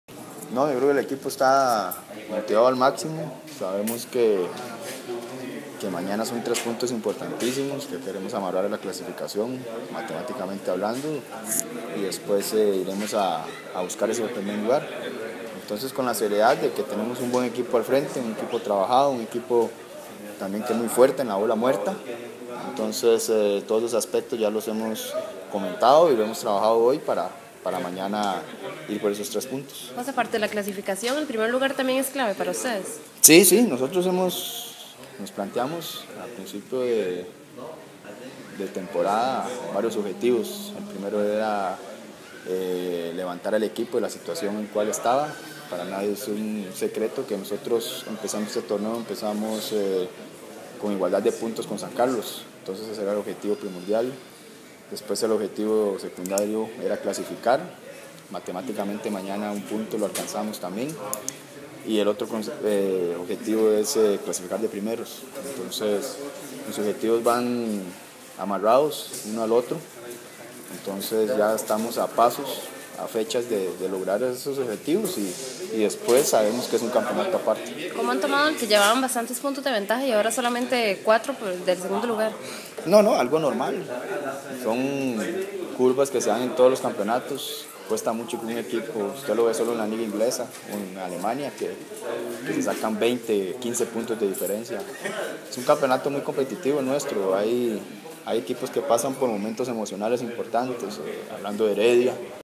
José Villalobos Chan: "El equipo está motivado al máximo"
El jugador del Club Sport Cartaginés habla del cierre de la fase regular del equipo